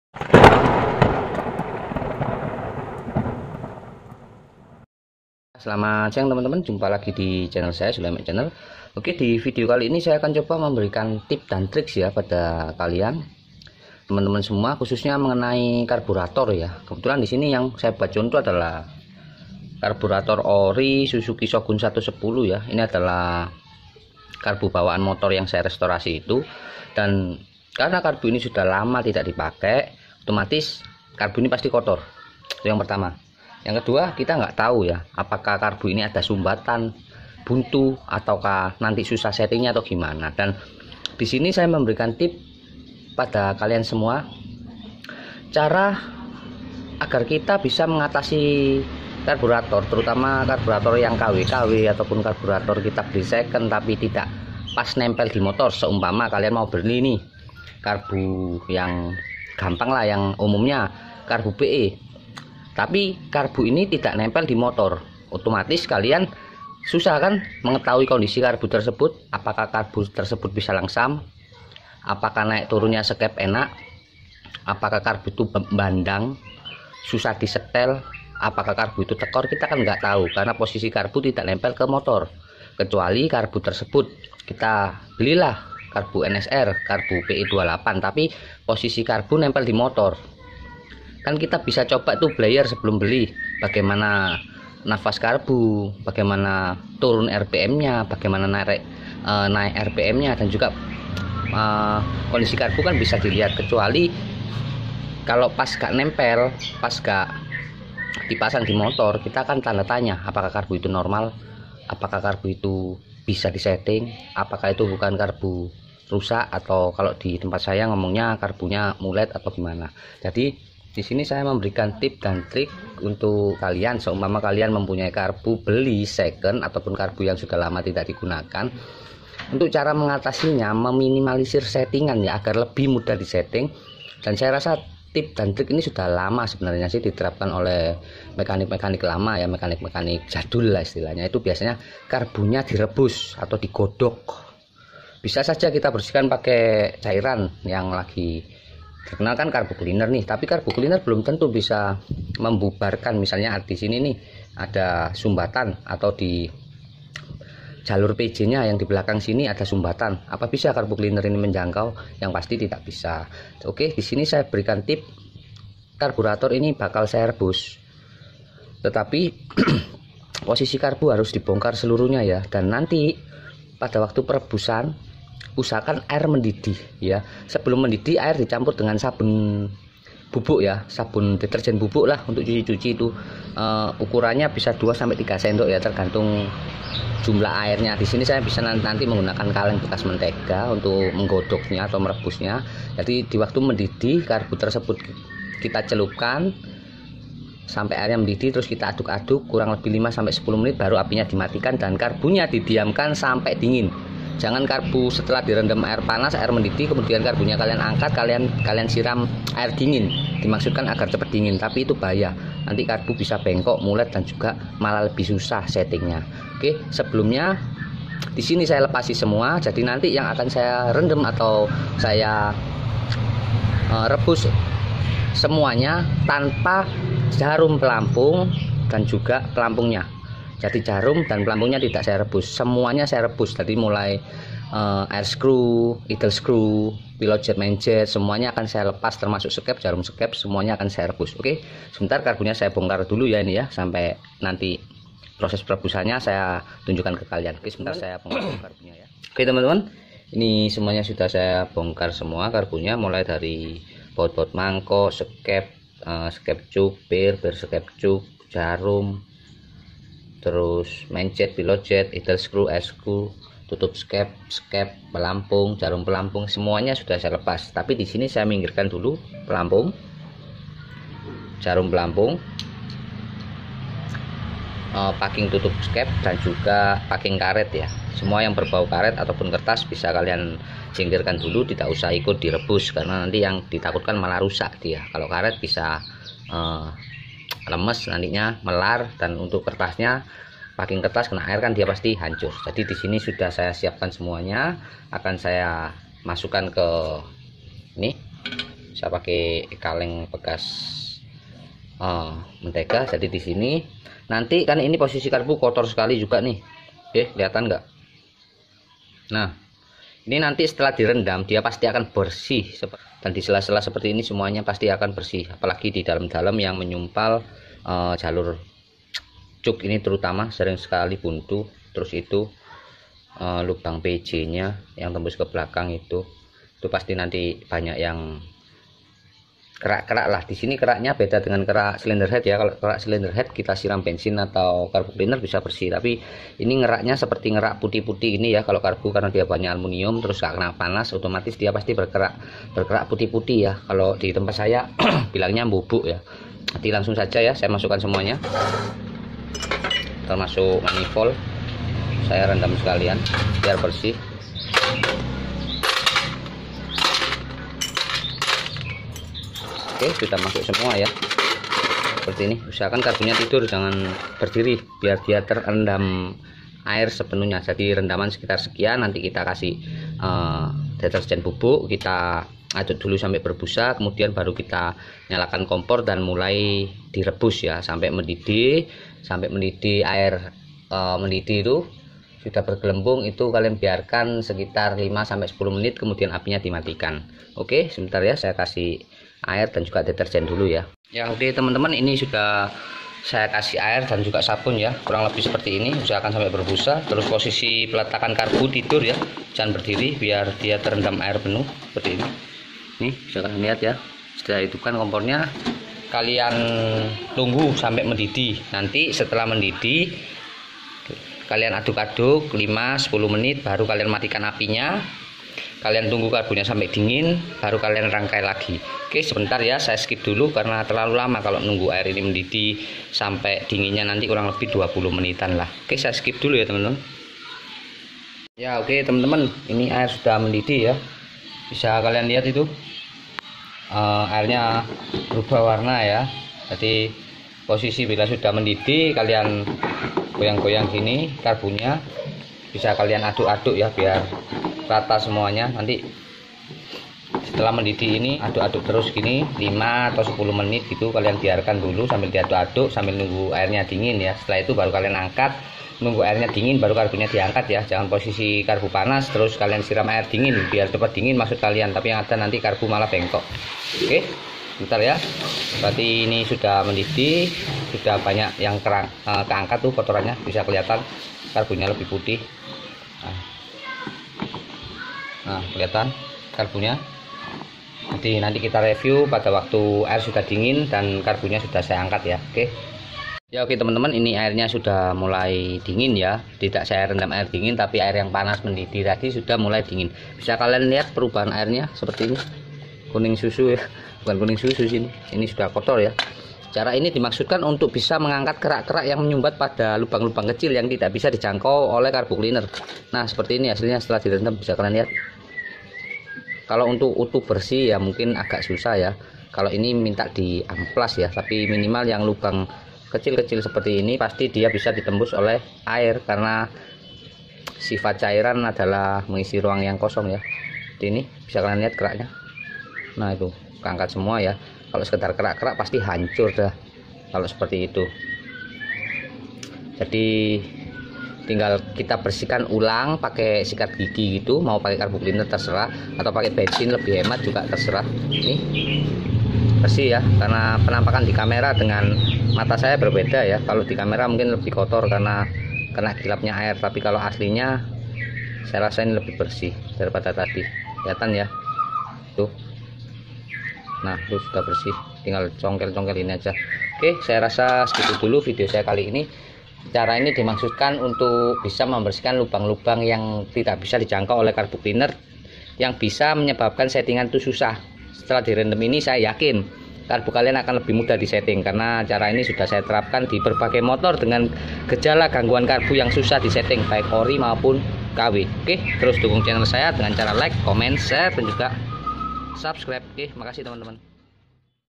Cara Mengatasi Karburator Yang Susah Langsam // Susah Disetting
Divideo kali ini saya akan coba memberikan tips n trik untuk mengatasi karburator yang susah disetting,gas susah turun,rpm susah naik ataupun masalah2 ringan pada saat menyetel karburator..
Simak videonya sampai habis ya..
Terimakasihh...